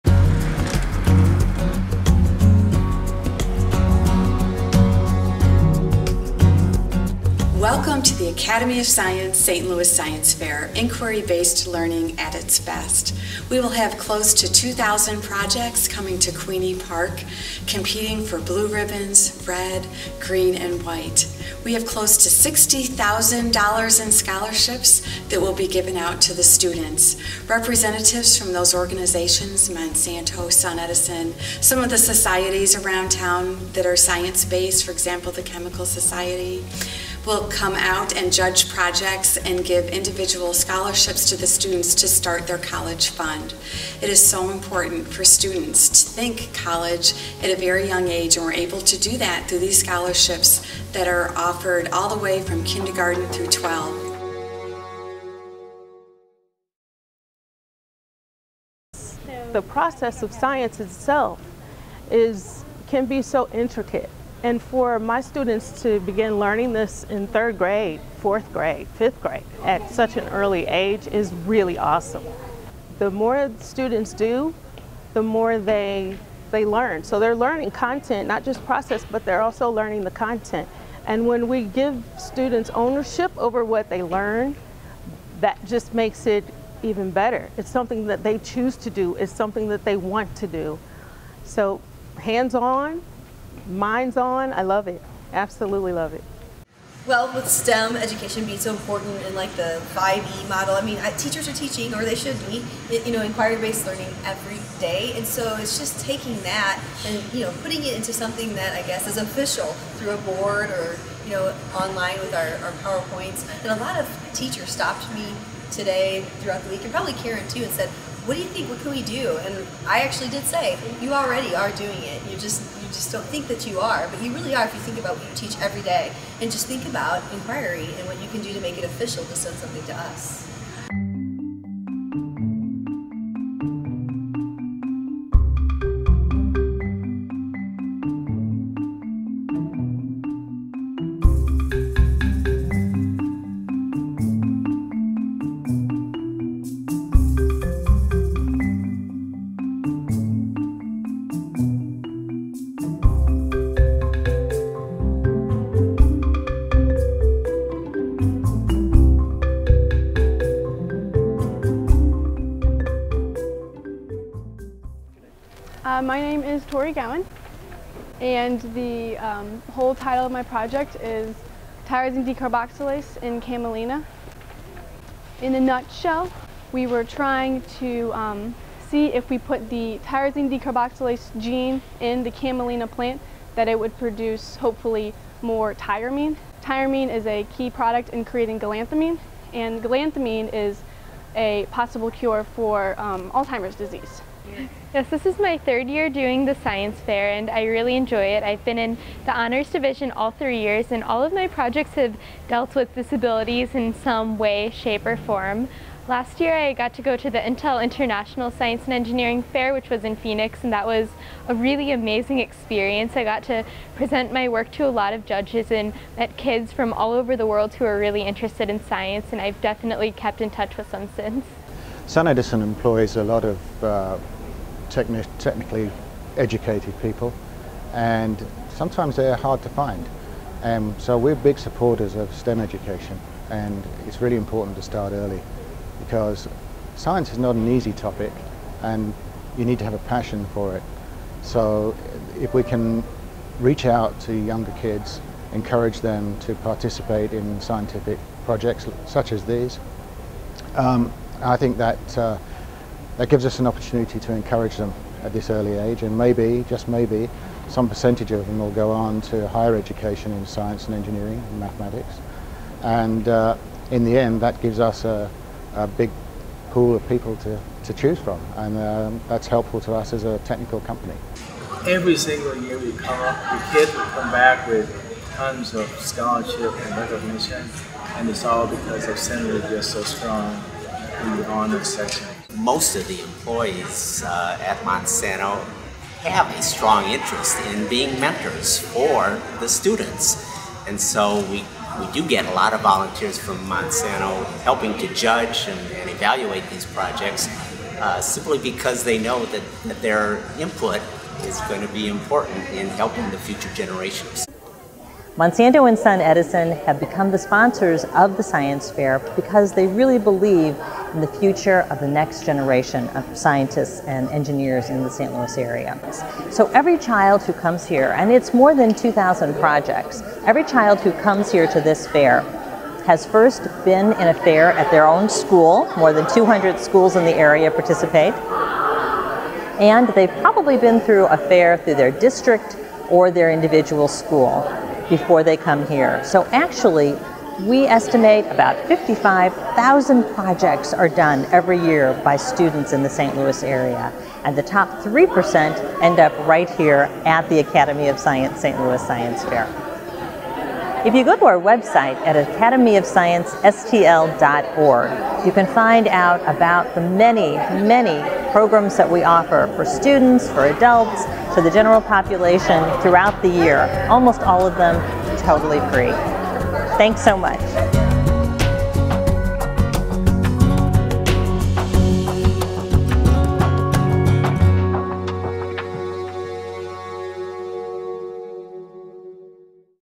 The Academy of Science - St. Louis Science Fair
Science Fair overview - perspective from teacher and students. College scholarship opportunities!